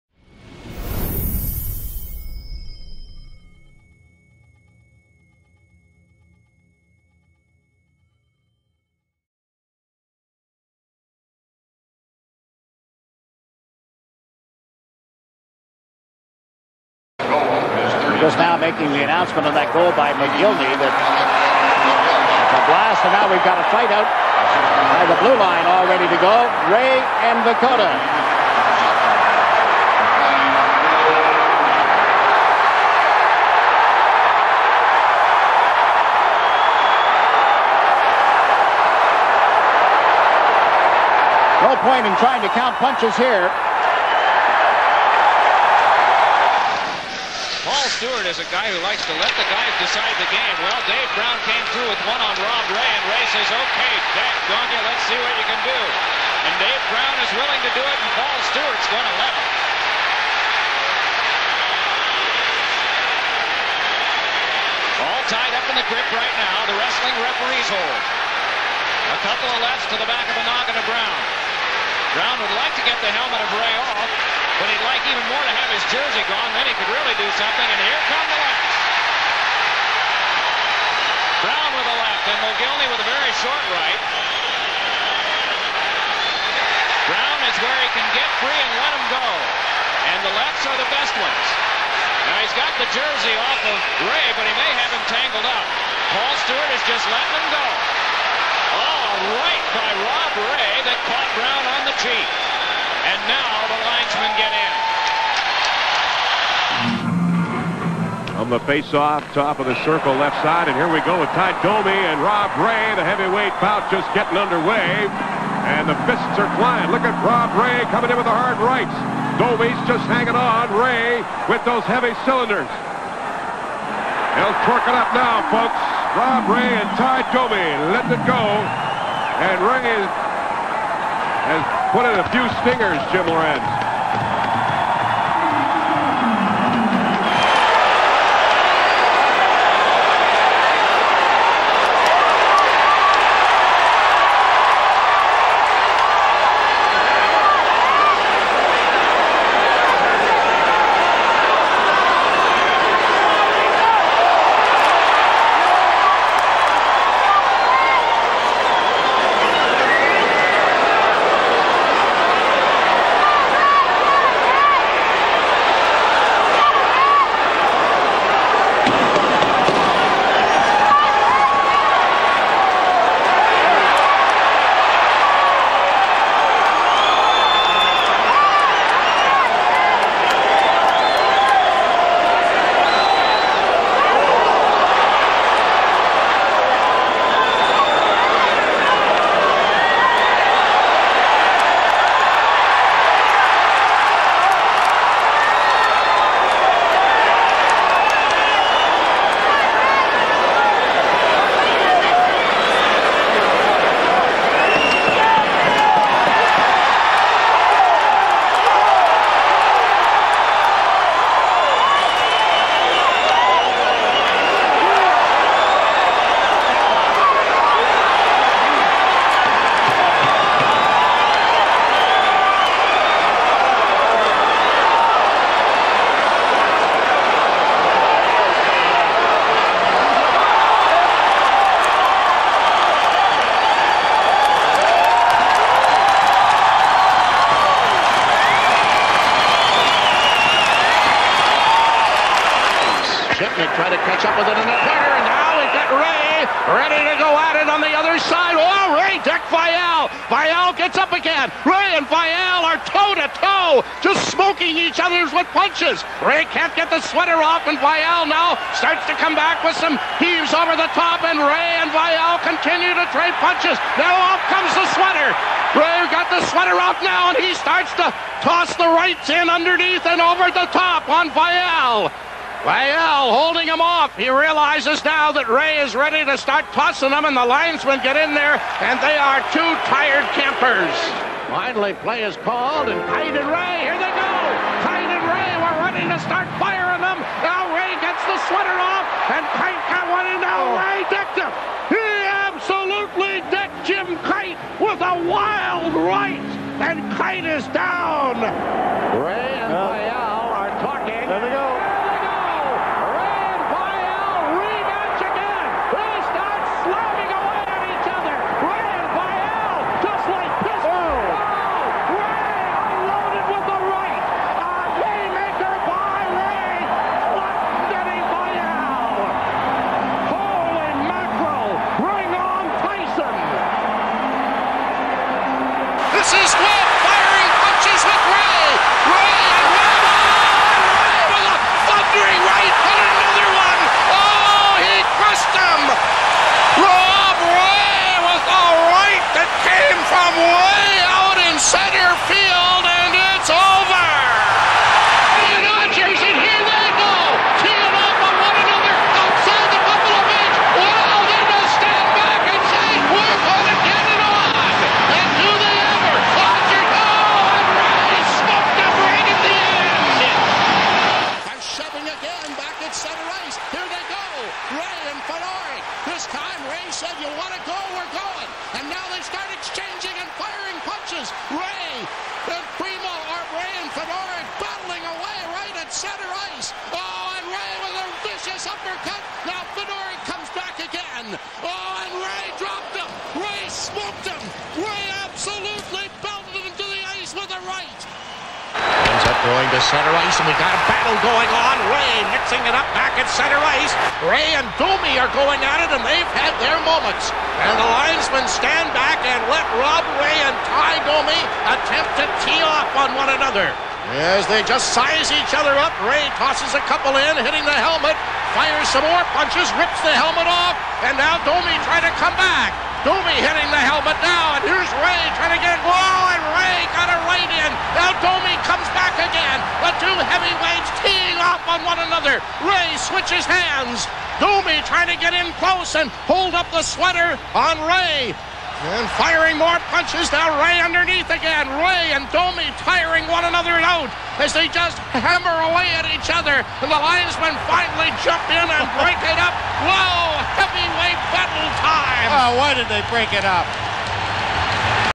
Top Ten NHL Hockey Fights of Rob Ray
1/8/1993 | Mick Vukota (Islanders)
1/26/1993 | Dave Brown (Philadelphia)
2/10/1993 | Tie Domi (Winnipeg)
11/19/1995 | Dennis Vial (Ottawa)
2/28/1996 | Dennis Vial (Ottawa)
3/10/1996 | Jim Kyte (San Jose)
4/10/1996 | Dennis Vial (Ottawa)
2/21/1997 | Steve Webb (Islanders)
3/26/2001 | Jeff Odgers (Atlanta)
1/7/2003 | Todd Fedoruk (Philadelphia)